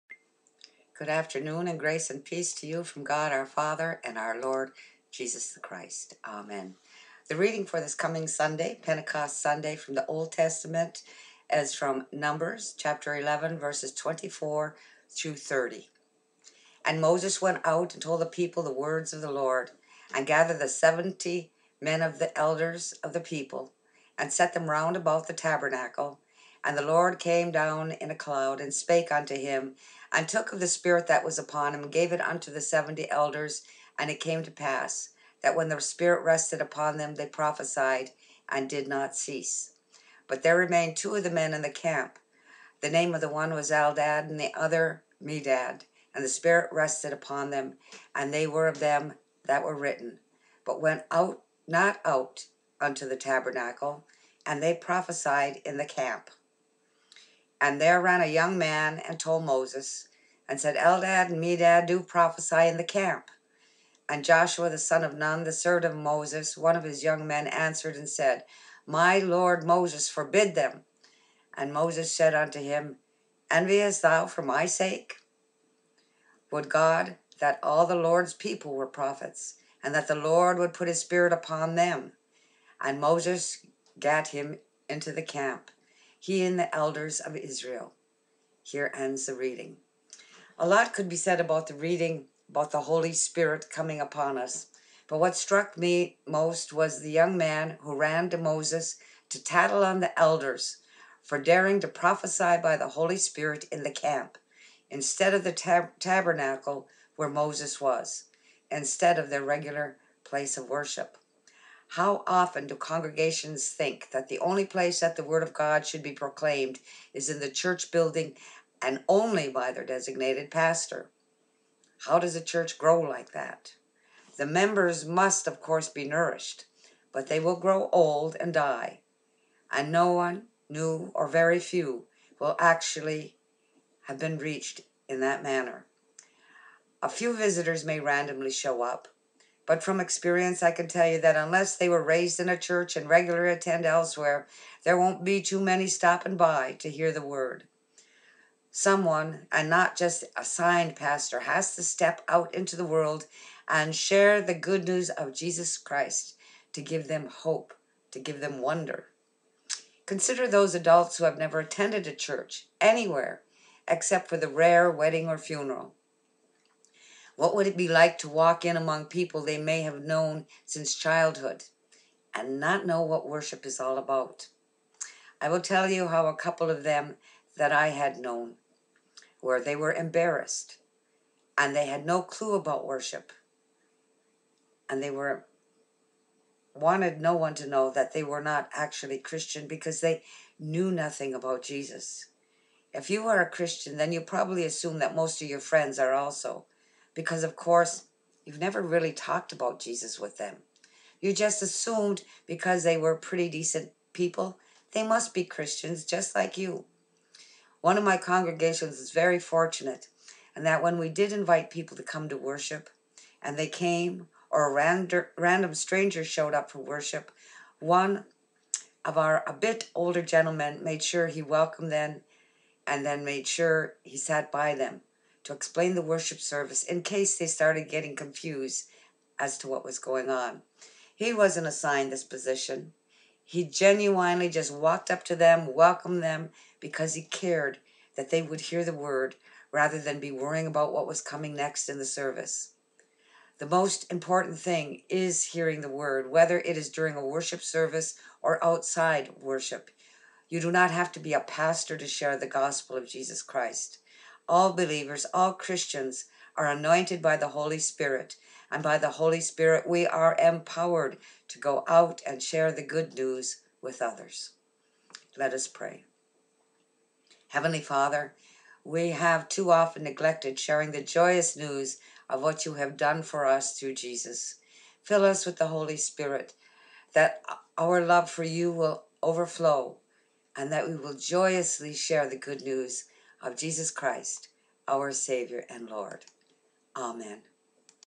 Not Just In The Temple
Numbers 11:24-30 KJV; OT Reading for Pentecost Sunday; Proclaim the Good News of Jesus everywhere.